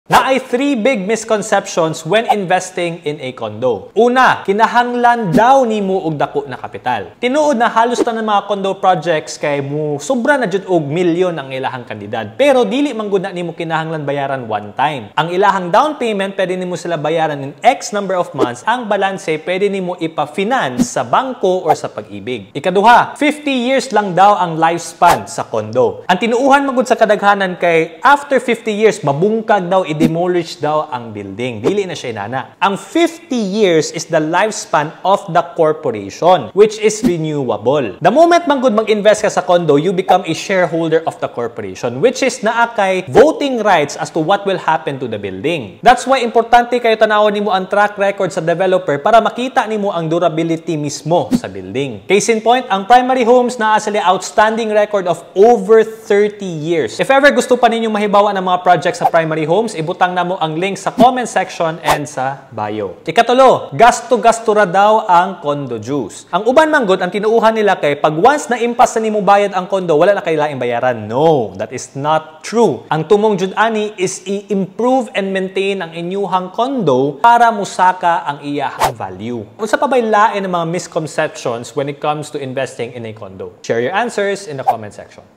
Condo Myths
3 big misconceptions when investing in a condo.